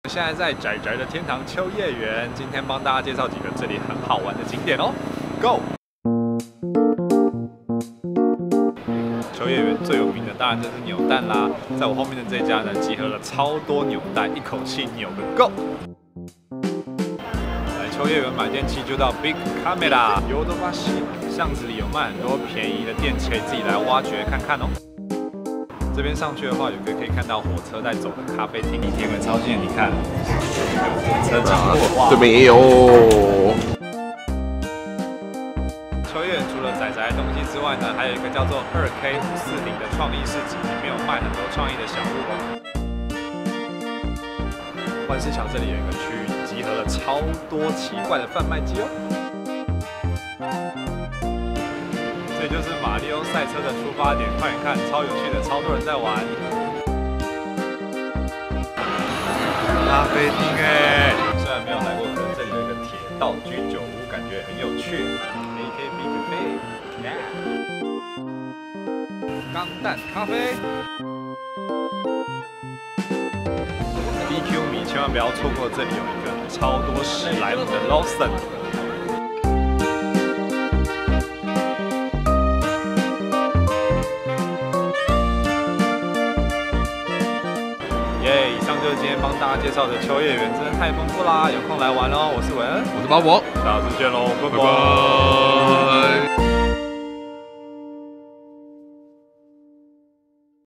秋葉原的多樣化景點
🤓跟著阿宅大叔玩秋葉原🤓
今天來到了宅宅天堂秋葉原，帶大家去《玩扭蛋》、《買電器》、《鐵道咖啡廳》、《逛創意市集》、《買怪奇販賣機》，還有太多說不完的東西，給我自己看影片！